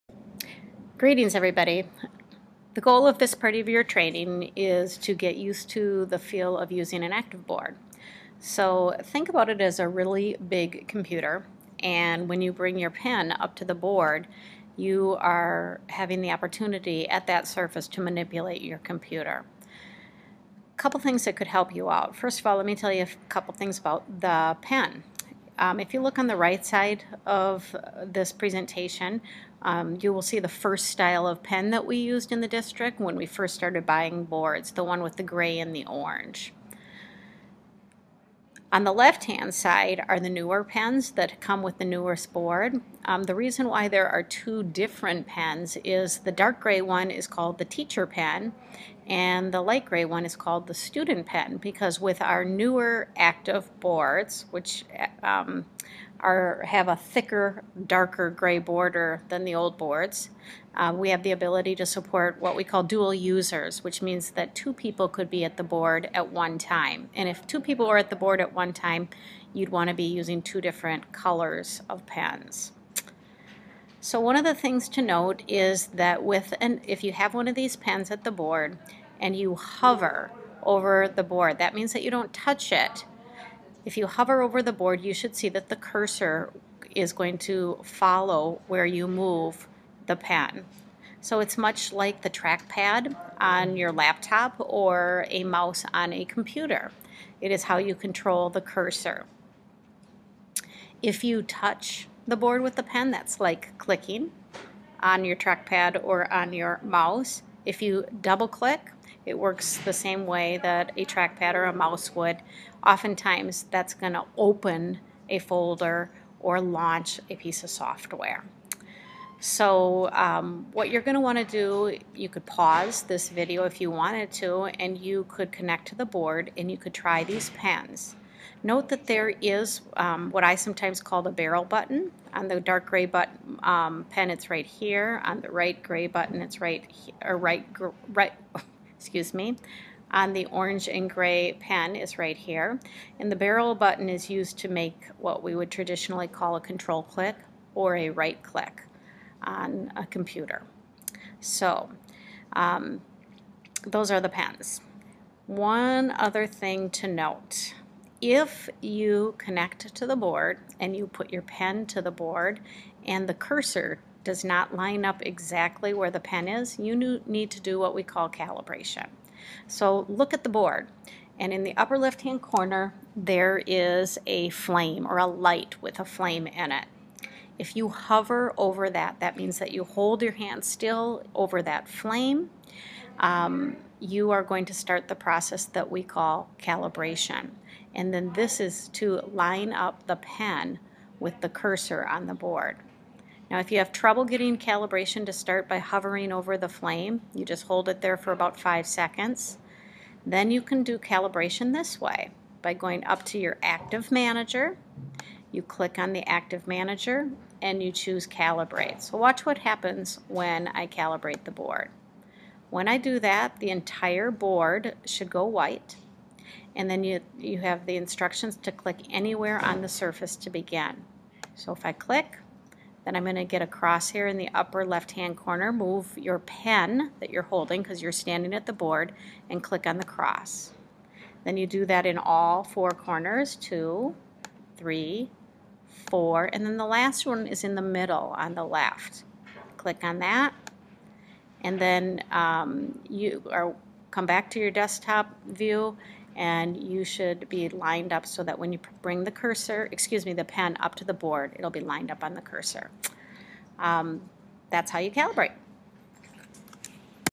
Calibration of the Activboard
A very basic beginning skill you need to have to successfully use an Activboard is the ability to calibrate or line up the pen with the cursor that appears on the board.  Learn how.